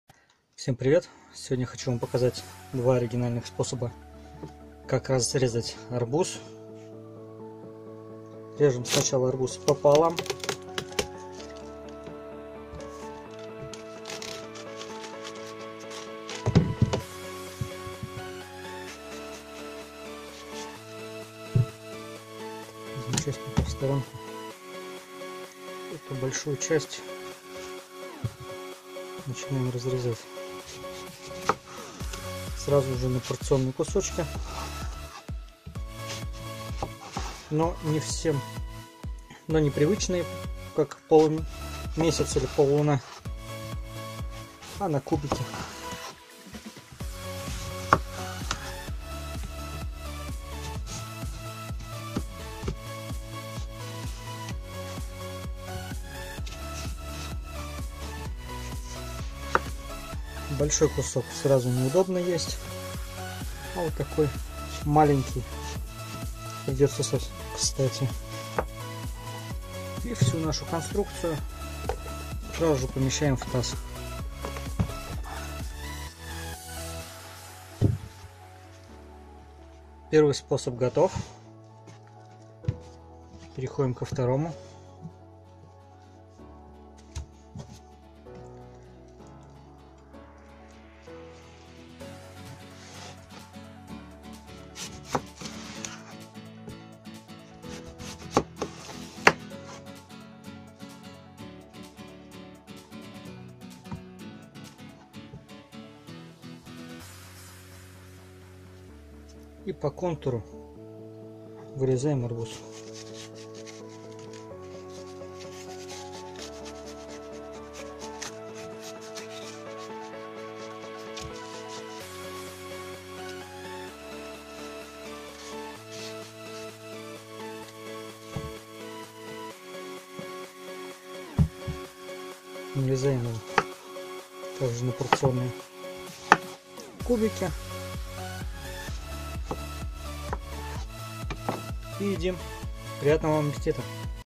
Арбузный лайфхак
В этом видео, я покажу 2 способа нарезки арбуза